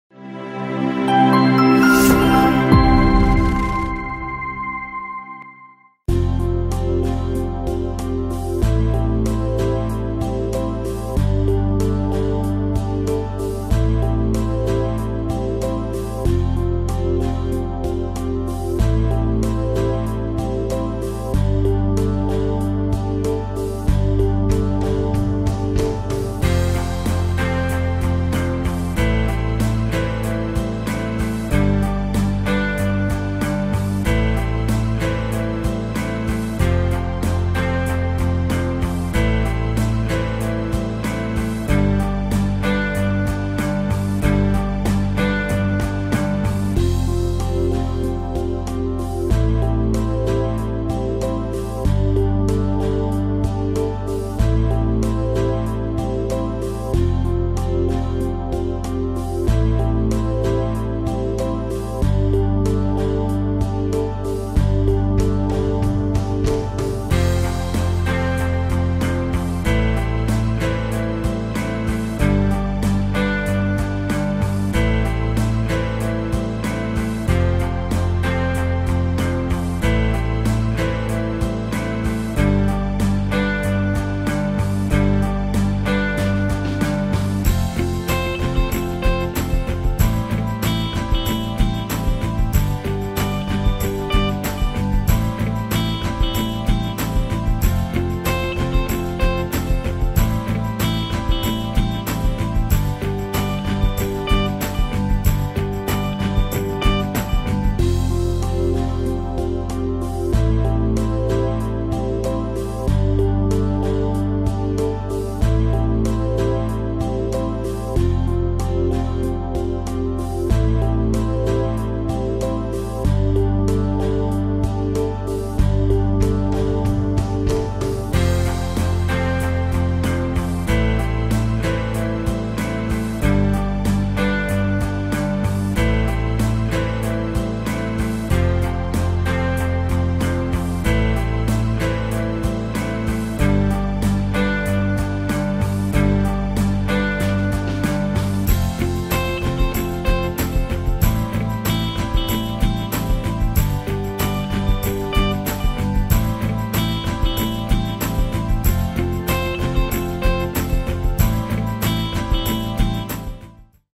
10 Marla Double Story House For Sale | Bahria Town Phase 8 Sector B
10 Marla Double Story House For Sale | Bahria Town Phase 8

Bahria Town Phase 8 Rawalpindi sector B 10 Marla Double story  used house for sale Price  Rs.16500,000 ,More detail . Habib elahi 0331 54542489a web